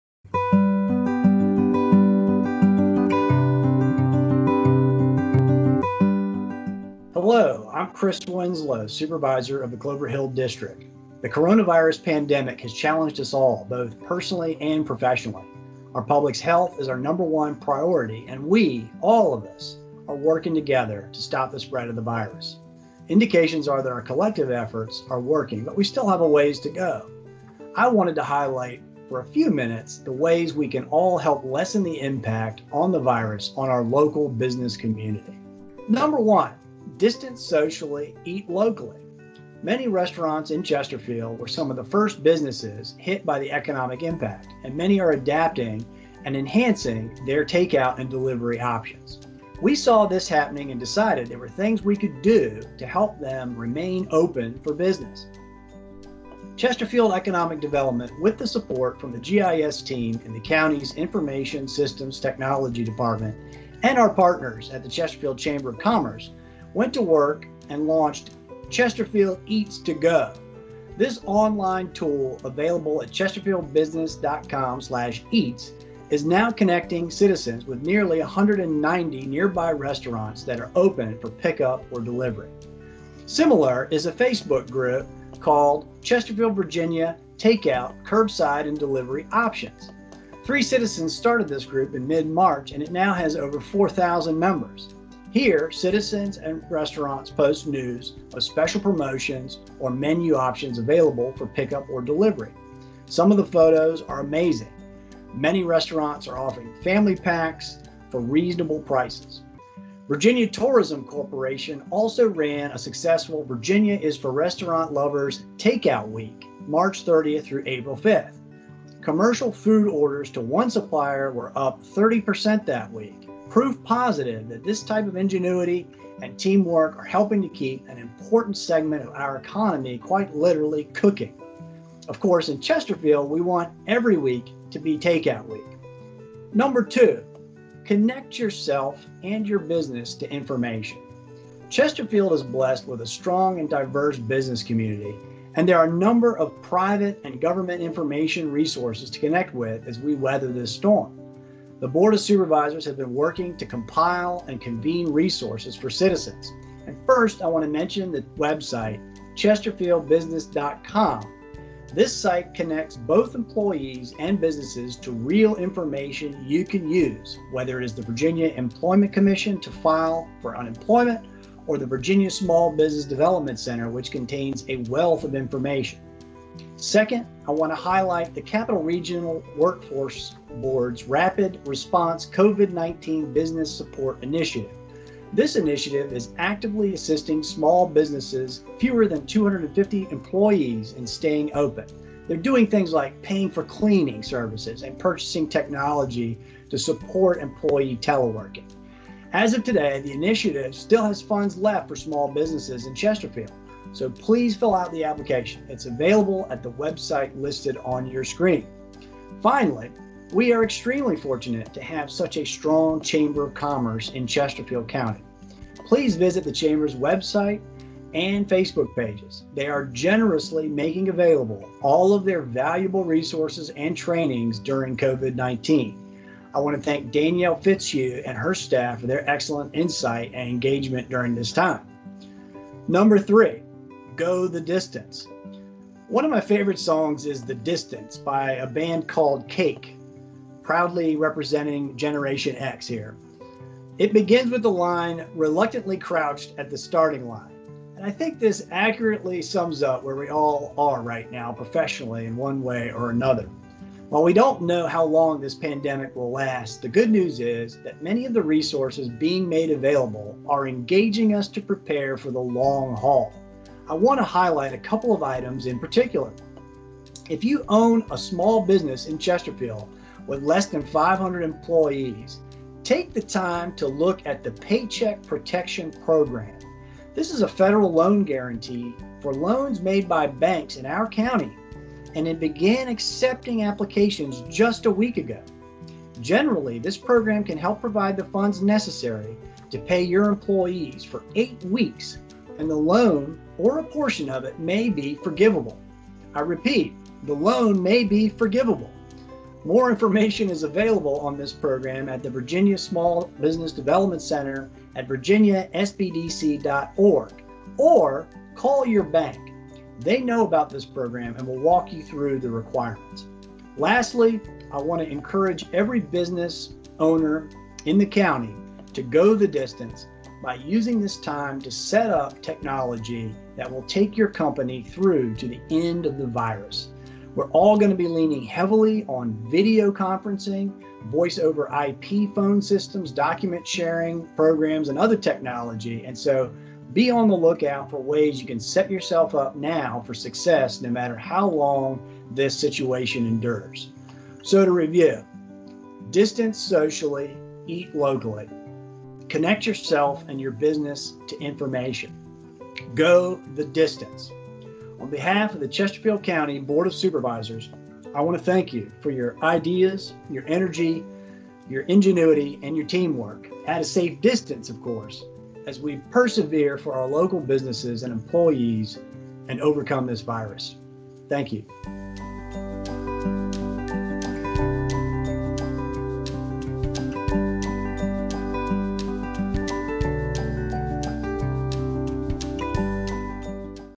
Chesterfield County Board of Supervisors Message - Chris Winslow
Chesterfield County Clover Hill District Supervisor Chris Winslow shares some of the resources available for businesses and citizens during the COVID-19 outbreak.